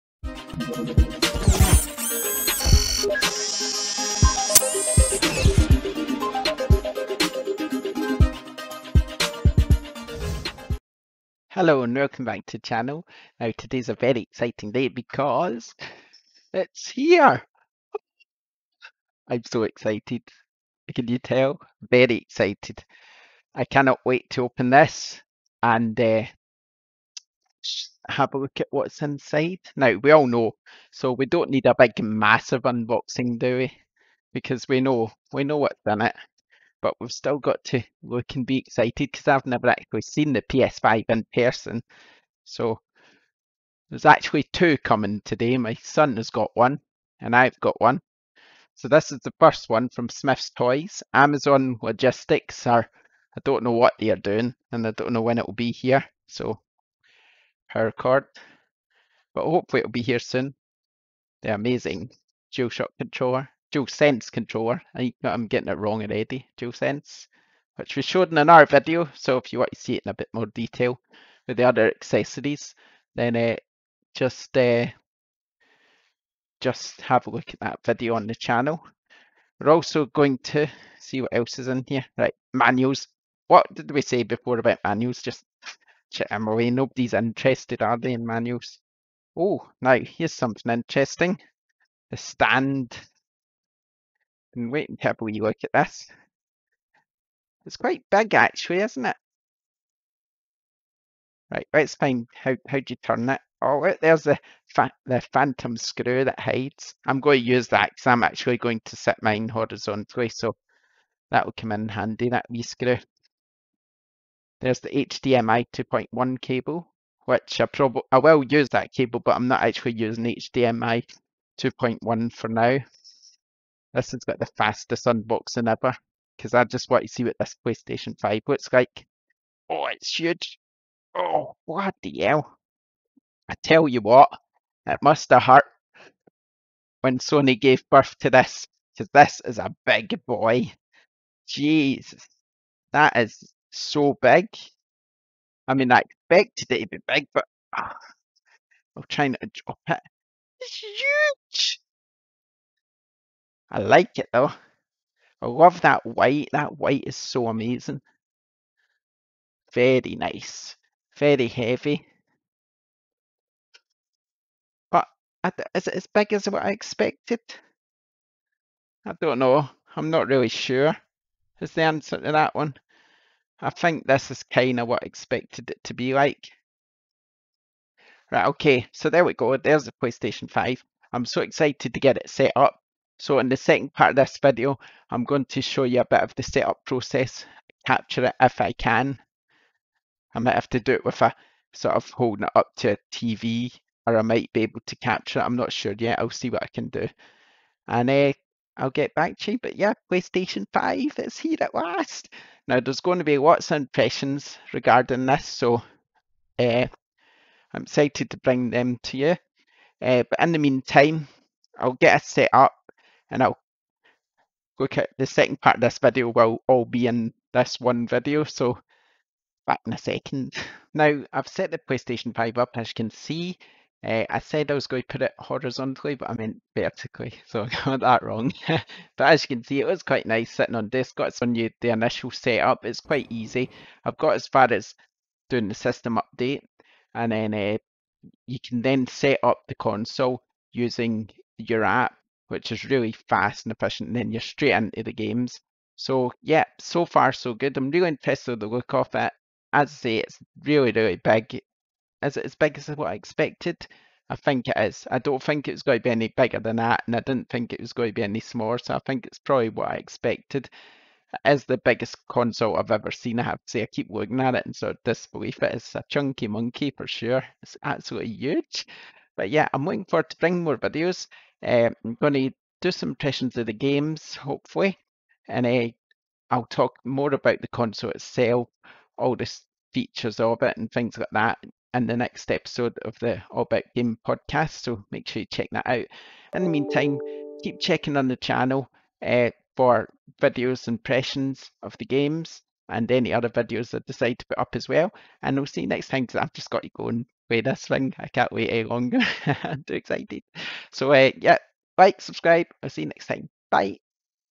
PS5 Unboxing Video - Far Too Excited Cut
The PlayStation 5 is here and to be honest, should probably have left this video for a day as I was far too excited.

Still, maybe this isn't such a bad thing. So let's have a look, shall we?